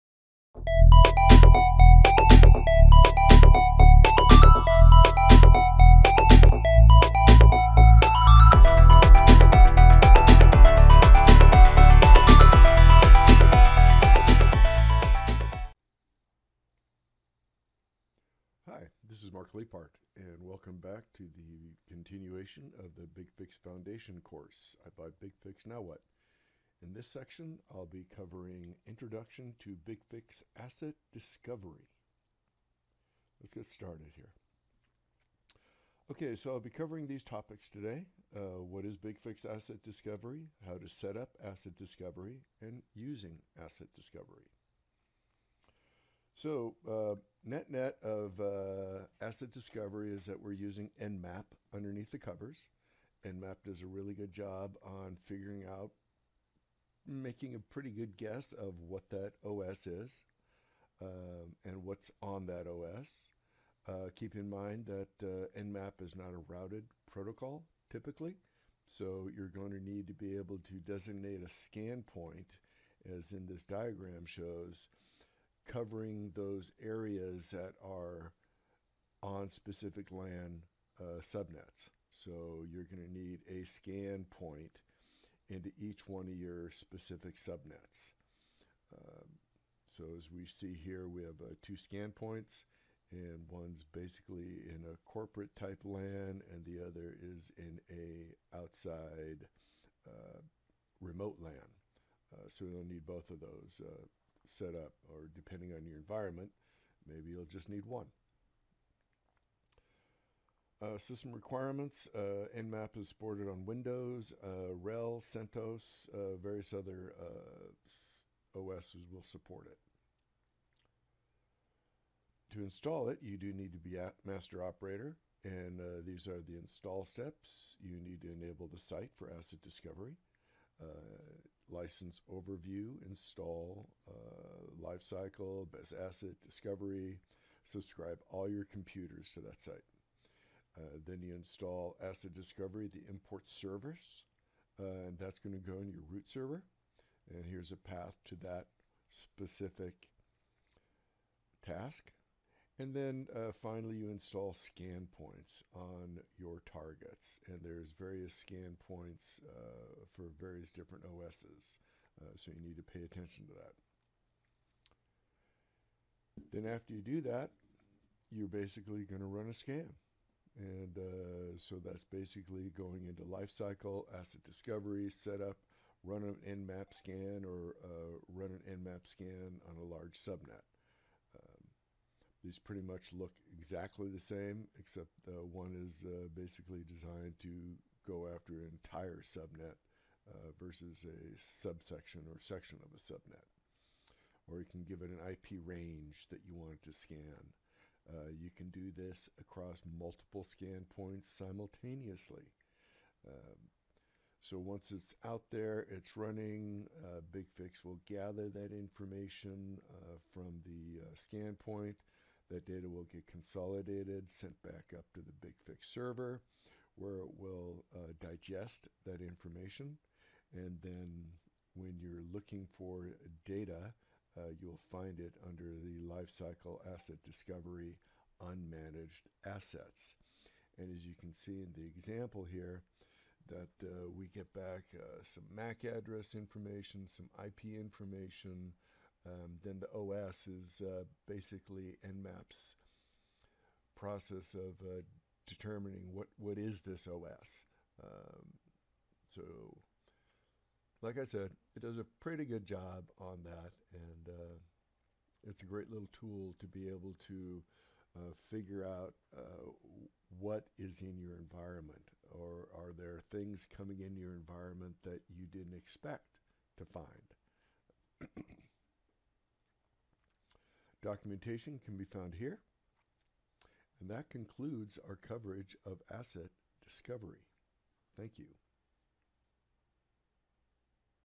BigFix Foundation Series - Asset Discovery
In this special series created by our Director of BigFix Enablement.  A variety of topics are covered to help a new user better understand BigFix.

This video provides an introduction to Asset Discovery